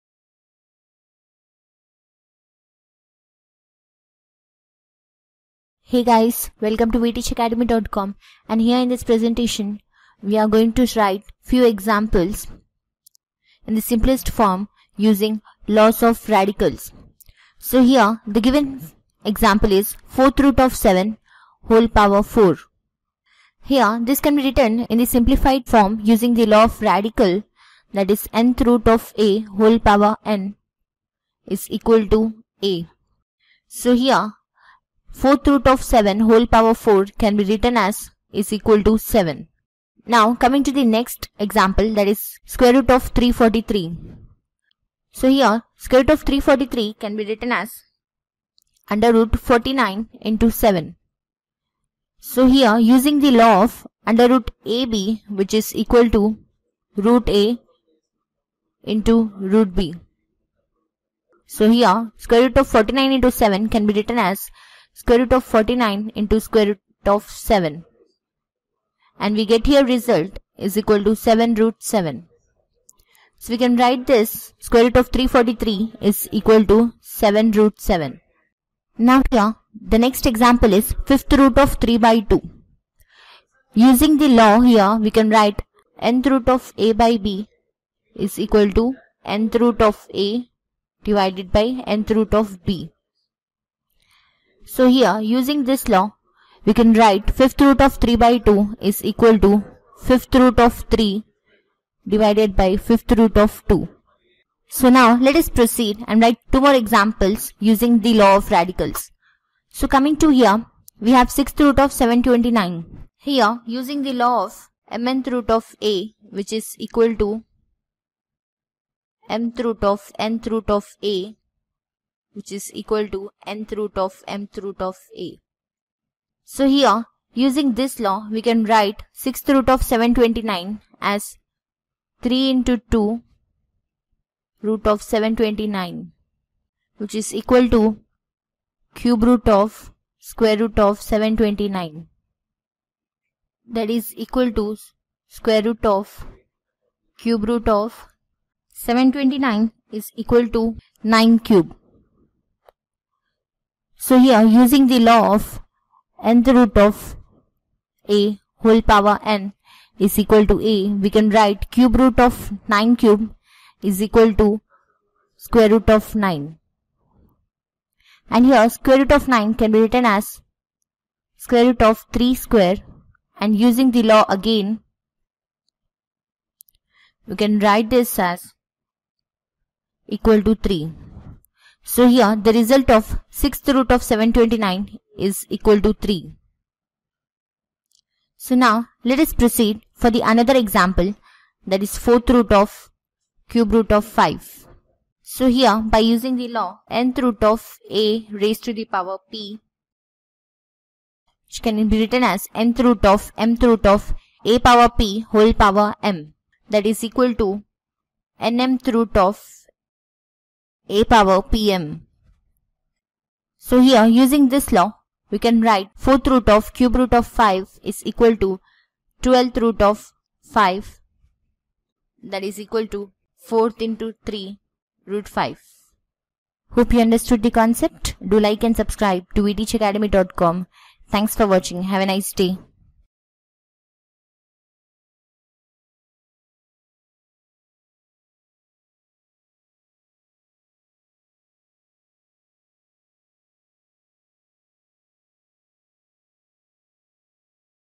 Basics Of Laws Of Radicals With Examples - Maths Arithmetic
What are the laws of radicals? Let us learn about the same with the help of some examples.